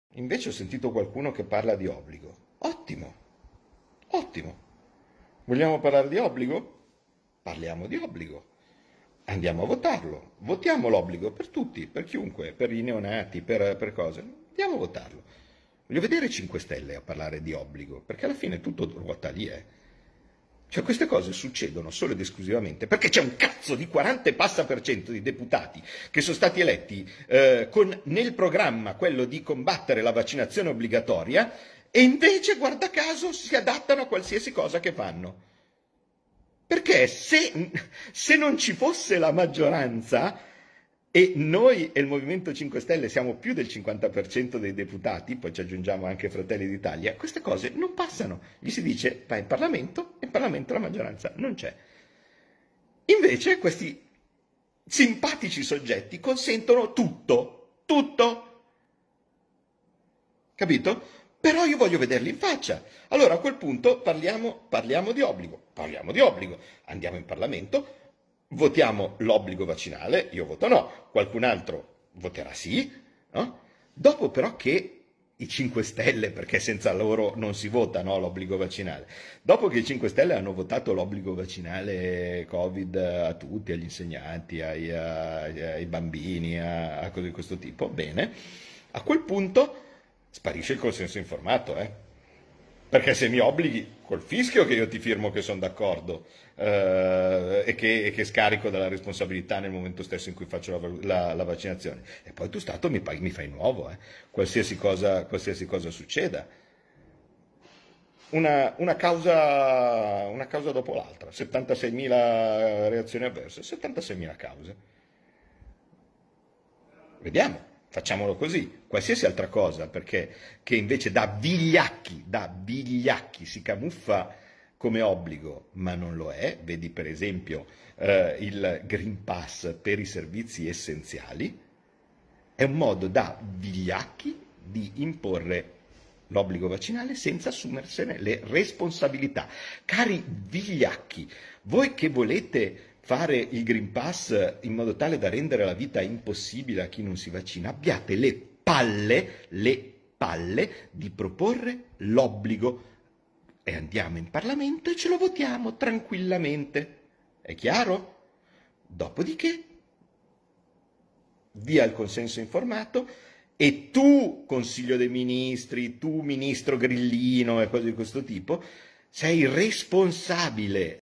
Green Pass? Meglio l'obbligo Vaccinale! Vi spiego il perchè.
Bisogna mettere le carte in tavola.
E Claudio Borghi lo fa: comodo parlare di sicurezza, responsabilità e coscienza civica, quando poi nessuno ci mette sul serio la faccia e non rischia nulla di suo.
In questo video l'Onorevole ci spiega perchè mettere l'obbligo vaccinale è più serio e sensato del Green Pass.
Facci sapere cosa ne pensi.